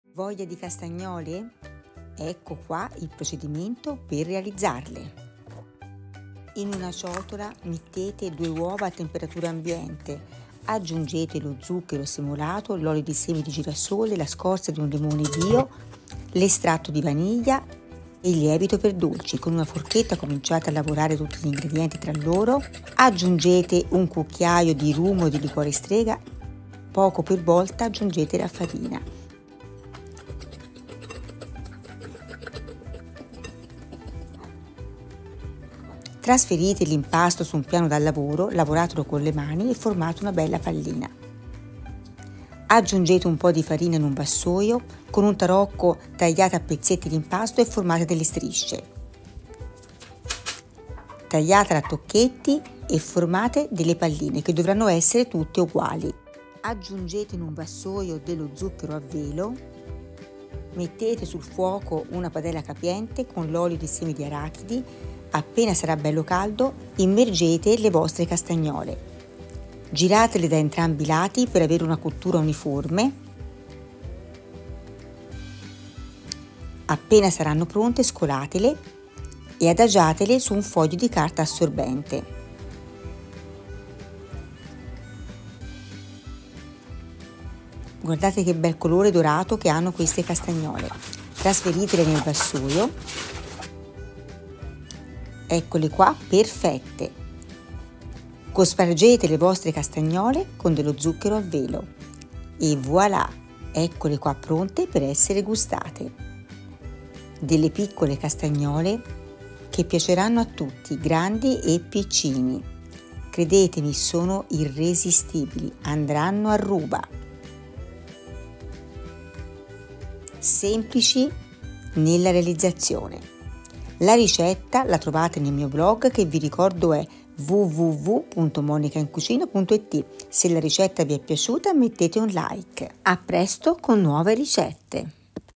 Le castagnole che tutti vorrebbero fare
Dei piccoli bocconcini golosi da realizzare per il periodo del carnevale . Credetemi una tira l’altra !
Ingredienti
350 gr farina 00
2 uova medie
75 gr zucchero semolato
75 gr olio di semi di girasole
8 gr lievito per dolci
1 cucchiaio di rum o anice o liquore strega
scorza di un limone 🍋 bio
1 cucchiaio di estratto di vaniglia (opzionale)
olio di arachidi per friggere q.b.